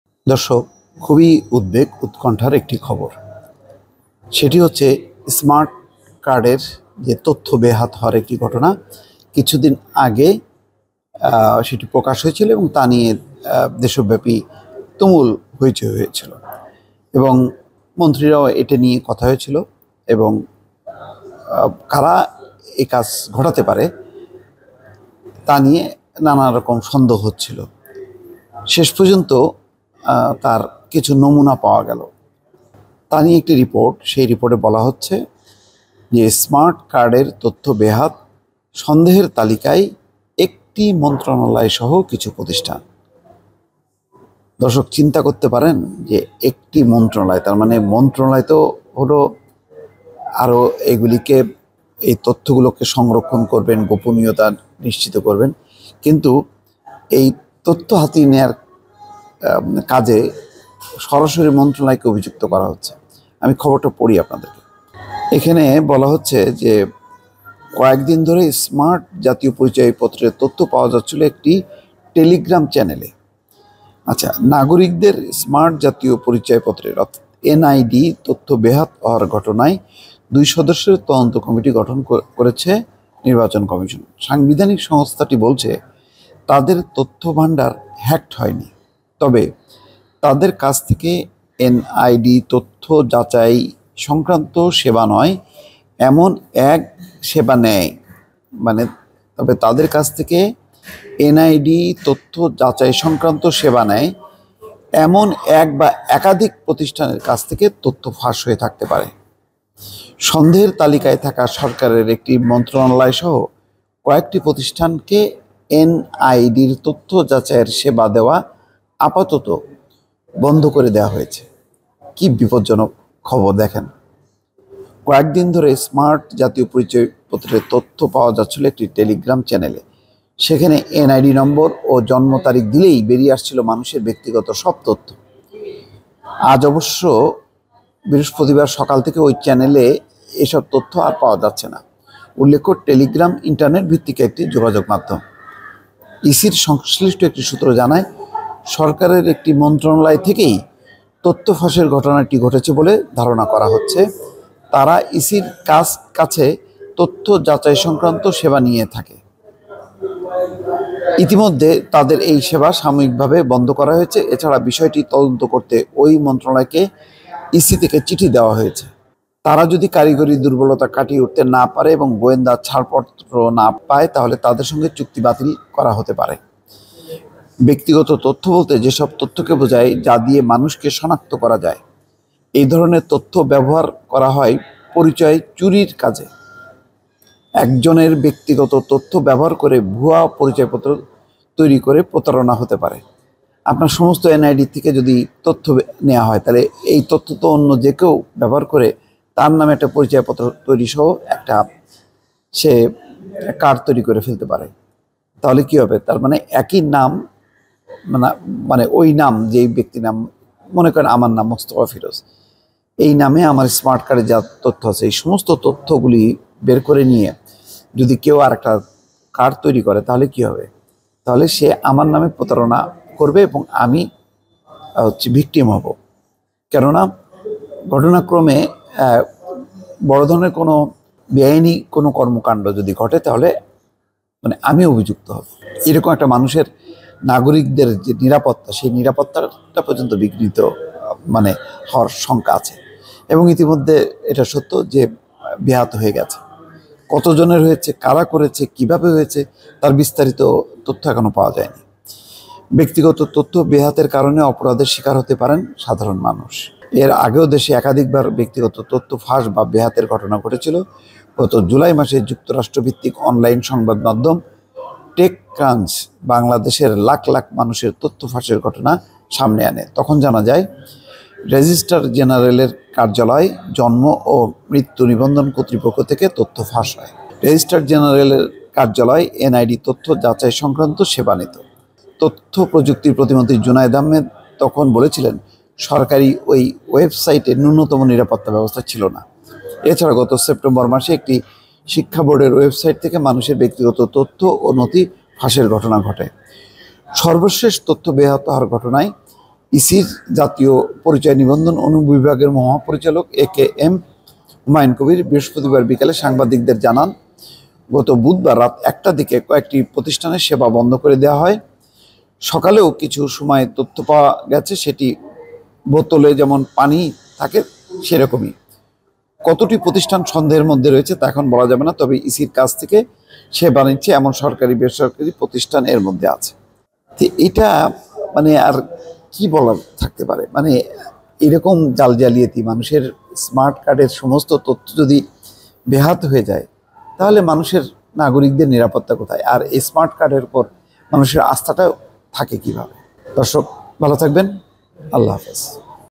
স্মার্ট কার্ডের তথ্য বেহাত....মন্ত্রণালয় অভিযুক্ত হলো এমন বেআইনি কাজে! Mostofa Feroz I Voice Bangla
স্মার্ট কার্ডের তথ্য বেহাত. মন্ত্রণালয় অভিযুক্ত হলো এমন বেআইনি কাজে!

*** ANTI-PIRACY WARNING ***